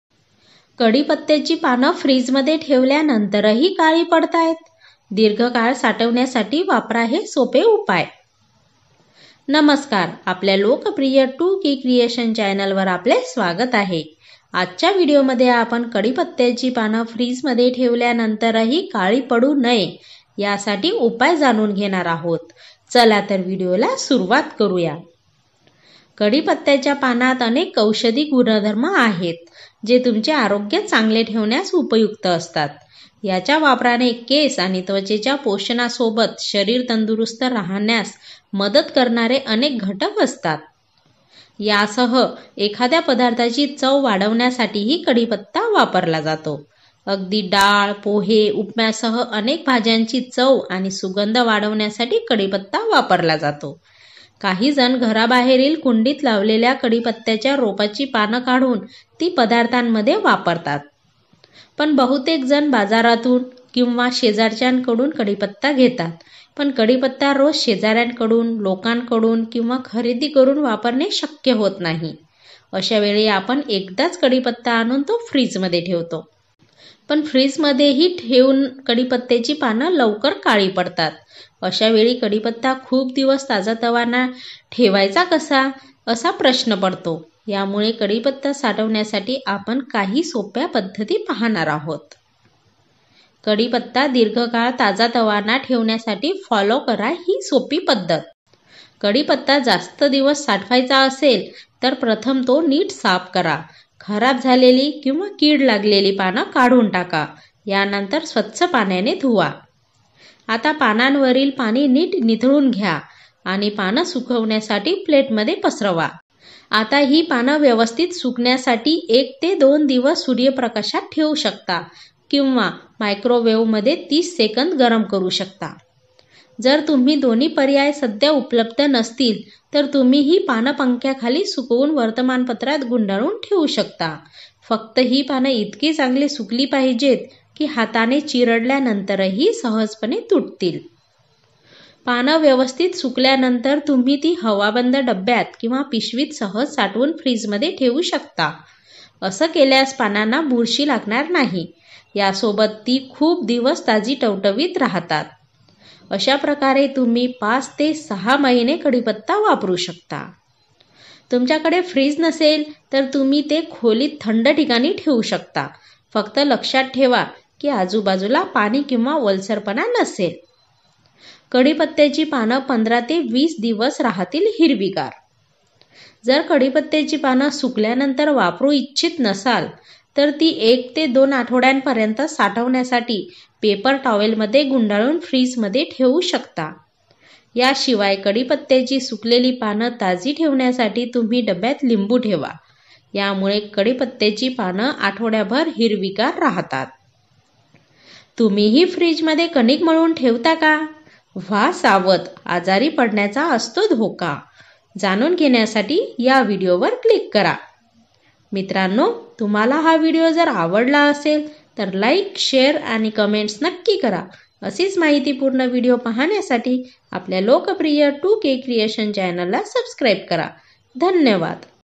कढीपत्त्याची पानं फ्रिजमध्ये ठेवल्यानंतरही काळी पडतायत? दीर्घकाळ साठवण्यासाठी वापरा ‘हे’ सोप्पे उपाय
नमस्कार, आपल्या लोकप्रिय "2kcreation" चॅनेल वर आपले स्वागत आहे. आजच्या व्हिडिओमध्ये आपण कढीपत्त्याची पानं फ्रिजमध्ये ठेवल्यानंतरही काळी पडू नये  यासाठी उपाय जाणून घेणार आहोत. चला तर व्हिडिओला सुरुवात करुया...

कढीपत्त्याच्या पानात अनेक औषधी गुणधर्म आहेत, जे तुमचे आरोग्य चांगले ठेवण्यास उपयुक्त असतात. याच्या वापराने केस आणि त्वचेच्या पोषणासोबत शरीर तंदुरुस्त राहण्यास मदत करणारे अनेक घटक असतात.

यासह एखाद्या पदार्थाची चव वाढवण्यासाठीही कढीपत्ता वापरला जातो. अगदी, डाळ, पोहे, उपम्यासह अनेक भाजांची चव आणि सुगंध वाढवण्यासाठी कढीपत्ता वापरला जातो. काहीजण घराबाहेरील कुंडीत लावलेल्या कढीपत्याच्या रोपाची पानं काढून ती पदार्थांमध्ये वापरतात.

पण बहुतेक जण बाजारातून किंवा शेजारच्यांकडून कढीपत्ता घेतात. पण कढीपत्ता रोज शेजाऱ्यांकडून लोकांकडून किंवा खरेदी करुन वापरणे शक्य होत नाही. अशावेळी आपण एकदाच कढीपत्ता आणून तो फ्रिजमध्ये ठेवतो.

 पण फ्रिजमध्येही ठेवूनही कढीपत्त्याची पानं लवकर काळी पडतात. अशावेळी कढीपत्ता खूप दिवस ताजातवाणा ठेवायचा कसा असा प्रश्न पडतो. यामुळे कढीपत्ता साठवण्यासाठी आपण
 काही सोप्या पद्धती पाहणार आहोत.

कढीपत्ता दीर्घकाळ ताजातवाणा ठेवण्यासाठी फॉलो करा ‘ही’ सोप्पी पद्धत

१) कढीपत्ता जास्त दिवस साठवायचा असेल तर प्रथम तो नीट साफ करा, खराब झालेली किंवा किड लागलेली पानं काढून टाका. यानंतर स्वच्छ पाण्याने धुवा.

२) आता पानांमधील पाणी नीट निथळून घ्या, आणि पानं सुकण्यासाठी प्लेटमध्ये पसरवा.

३) आता ही पानं व्यवस्थित सुकण्यासाठी १ ते २ दिवस सूर्यप्रकाशात ठेवू शकता. किंवा मायक्रोओव्हनमध्ये ३० सेकंद गरम करु शकता.

४) जर तुम्ही दोन्ही पर्याय सध्या उपलब्ध नसतील तर तुम्ही ही पानं पंख्याखाली सुकवून वर्तमानपत्रात गुंडाळून ठेऊ शकता. फक्त ही पानं इतकी चांगली सुकली पाहिजेत की हाताने चिरडल्यानंतरही सहजपणे तुटतील.

५) पानं व्यवस्थित सुकल्यानंतर तुम्ही ती हवाबंद डब्यात किंवा पिशवीत सहज साठवून फ्रिजमध्ये ठेवू शकता. अस केल्यास पानांना बुरशी लागणार नाही. यासोबत ती खूप दिवस ताजी टवटवीत राहतात. अशाप्रकारे तुम्ही ५ – ६ महिने कढीपत्ता वापरु शकता.

६) तुमच्याकडे फ्रीज नसेल तर तुम्ही ते खोलीत थंड ठिकाणी ठेवू शकता. फक्त लक्षात ठेवा की, आजूबाजूला पाणी किंवा ओलसरपणा नसेल.

कढीपत्त्याची पानं १५ – २० दिवस राहतील हिरवीगार

१) जर कढीपत्त्याची पानं सुकल्यानंतर वापरु इच्छित नसाल तर ती १ ते २ आठवड्यापर्यंत साठवण्यासाठी पेपर टॉव्हेलमध्ये गुंडाळून फ्रिजमध्ये ठेवू शकता.

२) याशिवाय कढीपत्त्याची सुकलेली पानं ताजी ठेवण्यासाठी तुम्ही डब्ब्यात लिंबू ठेवा. यामुळे कढीपत्त्याची पानं आठवडाभर हिरवीगार राहतात.

तुम्हीही फ्रीजमध्ये कणीक मळून ठेवता का ? व्हा सावध, आजारी पडण्याचा असतो धोका जाणून घेण्यासाठी या व्हिडिओ वर क्लिक करा.

मित्रांनो, तुम्हाला हा व्हिडिओ जर आवडला असेल तर 'Like', 'Share' आणि Comments नक्की करा.

धन्यवाद